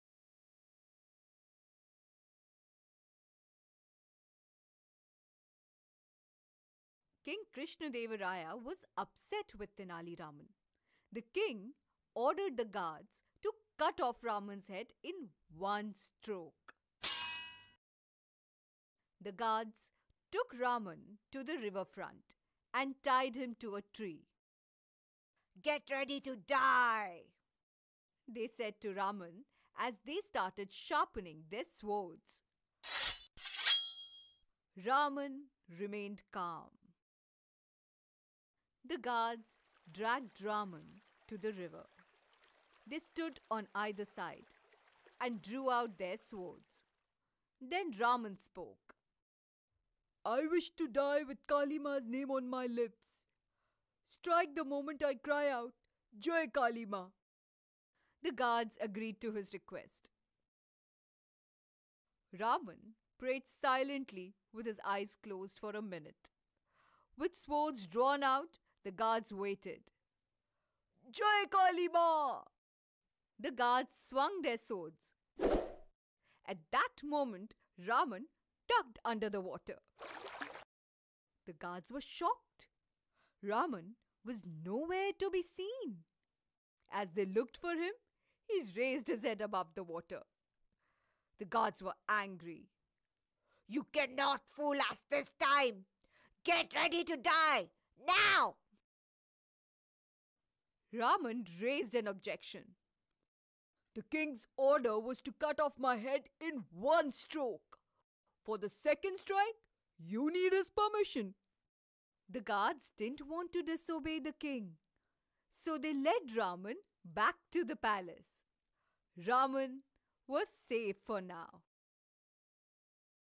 Tenali Raman's intelligency // save his life from soldiers// story story story story story story
Tenali Raman came to Hampi, the capital of Vijayanagara empire. At that time, Vijayanagara was ruled by Krishna Deva Raya.

Raman asked his wife to wait for him in a temple. “I’ll go to the palace and meet the king. He is a generous man. He may give me some gifts,” said Raman.

When Raman reached the palace, he was stopped at the entrance. “You cannot enter the palace without permission,” said the guard.

“I’ve come from a far-off place. Please allow me to enter the palace,” said Raman. “I’m hoping that the generous king will give me gifts,” he added.

“You may get gifts from the king. But what do I get?” asked the guard giving Raman a meaningful look.

“Whatever the king gives me, I’ll share it with you,” Raman promised.

“Half of it. Whatever gifts the king will give you, you must give me half of it,” said the guard. Raman agreed and walked into the palace.

In the palace, the king was holding a durbar in a hall. Raman was stopped at the entrance to the hall by another guard. Raman promised to give the second guard half of the gifts he would receive from the king.

When Raman walked in, the king was not in a good mood. Raman was very excited to be in the presence of the king; he rushed forward. The king was annoyed because no one could approach him without being called.

“Give him fifty lashes,” ordered the king.

Raman folded his hands in salutation to the king and said, “I’m grateful to you for your generous gift. Please allow me to share the gifts with two of my friends.”

The king wanted to know who his friends were.

“The guard at the entrance to this hall and the other one at the main entrance to the palace,” said Raman.

The guards were summoned. The king was quite annoyed with his guards. “Now that I know you two are sharing the ‘gift’, I’ll increase it to one hundred lashes,” said the king.

The guards were taken away to be given fifty lashes each.

“You wait here, until your friends return,” said the king to Raman.

When the guards returned after receiving their shares of the ‘gift.,’ the king turned to Raman.

“You have tempted my honest guards with bribes. I give severe punishment to bribe givers,” said the king sternly.

The king turned to the two guards. “Take him to the river side and cut off his head with a single stroke from your swords,” the king ordered.

The guards were happy to have their revenge. They led Raman to the river front.

Get notified of the best stories right in your inbox!

Tenali Raman went to Krishna Deva Raya, the king of Vijayanagar, hoping to receive some gifts. The gift he got was a hundred lashes for approaching the king without an appointment! Raman passed on the ‘gift’ to the two guards who had demanded half of the ‘gift’ he was going to collect. Of course, the two guards did not know that Raman would be rewarded fifty lashes!

The king, however, wanted to punish the Raman for bribing the guards. He ordered the guards to take Raman to the river front and cut off his head with a single stroke from their swords.

The guards who had received 50 lashes each had a score to settle with Raman. They were only too happy to be his executors.

They took him to the river front and tied him to a tree. Then they started sharpening their swords. An argument arose on who would execute Raman. Raman pointed out the king’s order was that he should be executed by both the guards. The guards agreed, untied Raman and dragged him to the river.

Standing on either side of Raman in the knee-deep water, the guards asked Raman to get ready to be executed. Raman said, “You do your duty. But I have one request. My only desire is to die with the name of Kaali Ma on my lips.”

“All right,” said the guards, “You pray. We’ll wait.”

“You be ready with your swords drawn out. Strike the moment I cry out, ’Jai Kaali Ma’,” requested Raman.

The guards agreed. Raman stood in water,

 annoyed with him. “You cannot fool us this time. Get ready to die. Now.”

Raman raised an objection. “The king’s order was to cut off my head with one stroke which you have already used. For a second strike, you need the king’s permission.”